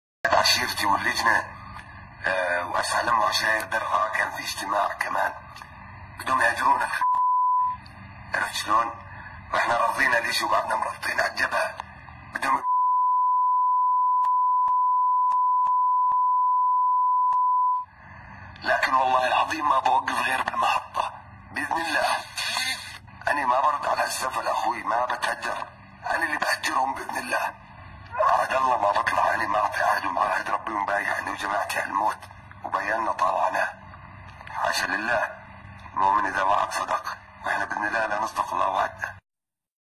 تسجيلات صوتية تثبت رفض حلول الدولة السورية لحفظ الأمن والأمان في درعا البلد
على رأسهم المسلح الملقب بـ هفو